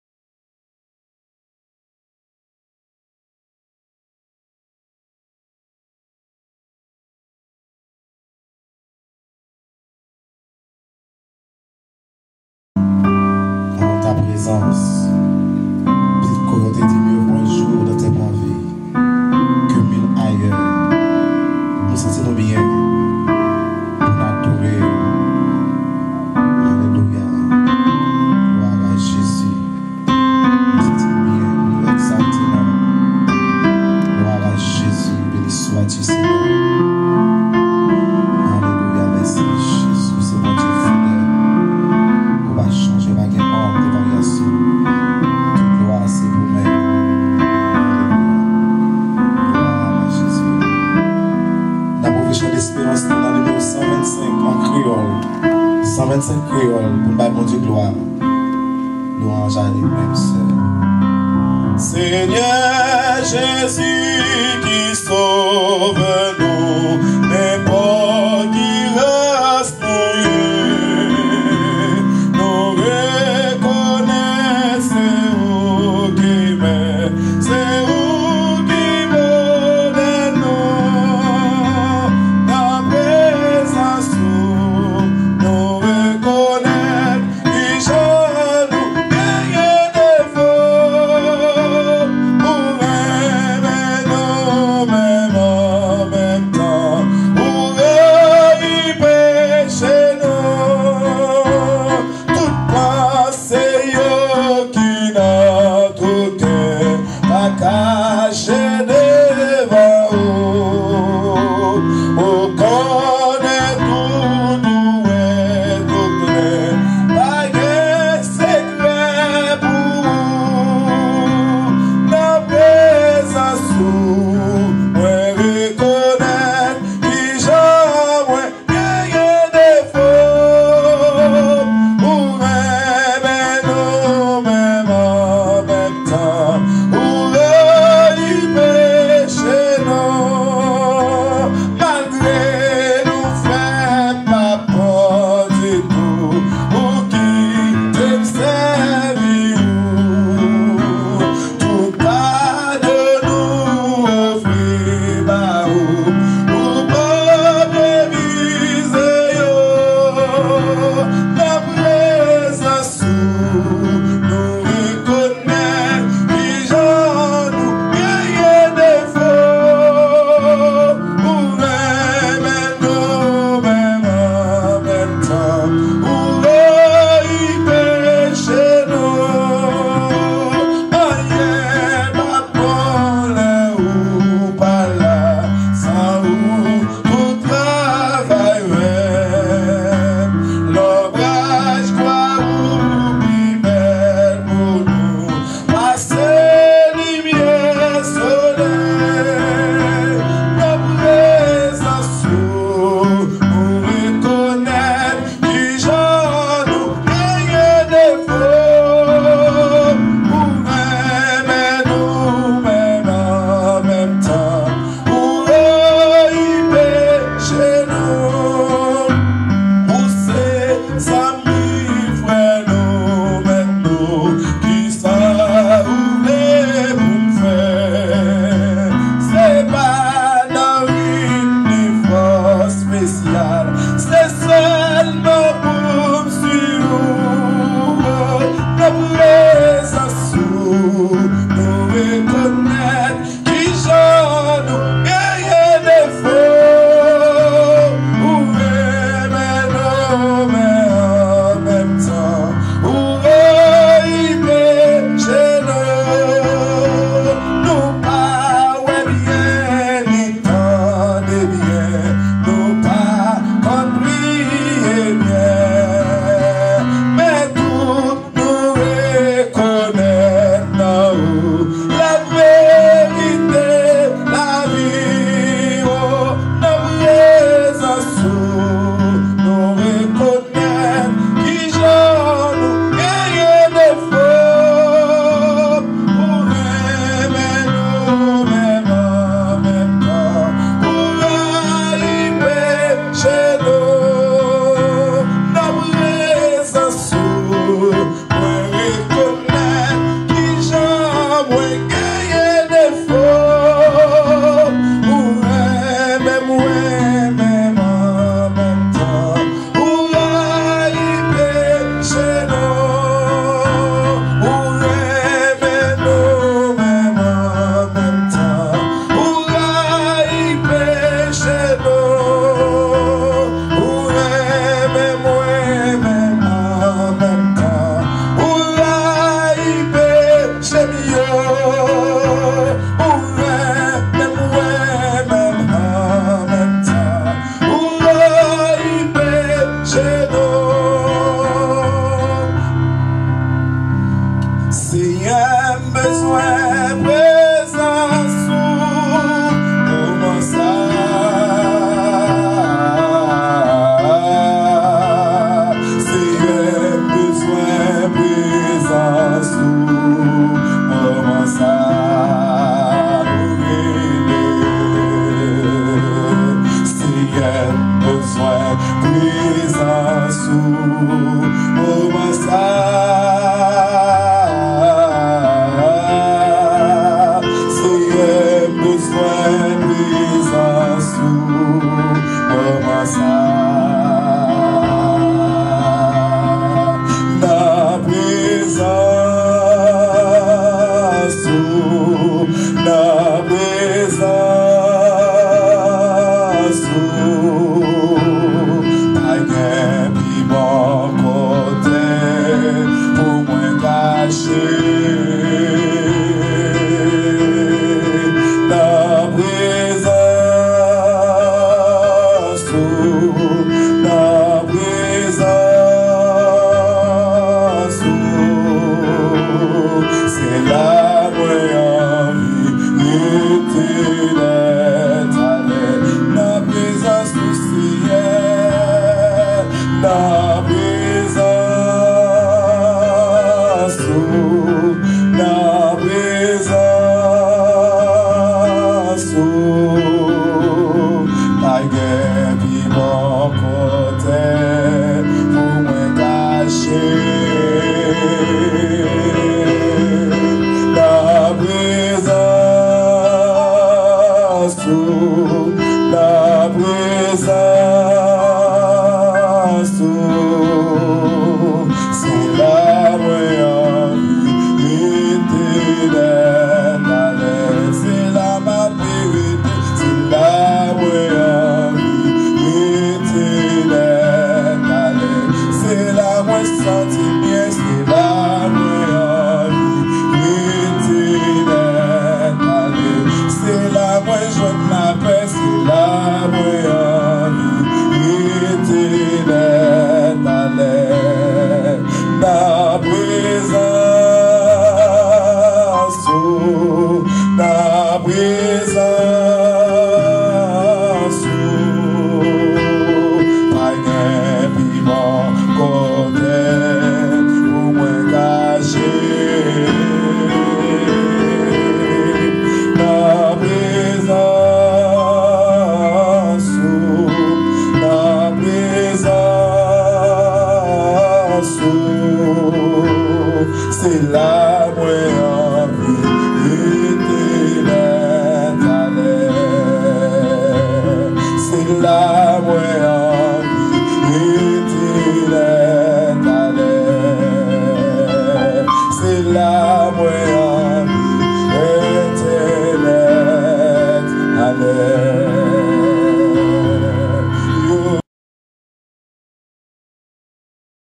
Michelet Nicolas
Michlet Nicolas Worship God at New Generation Church in Phily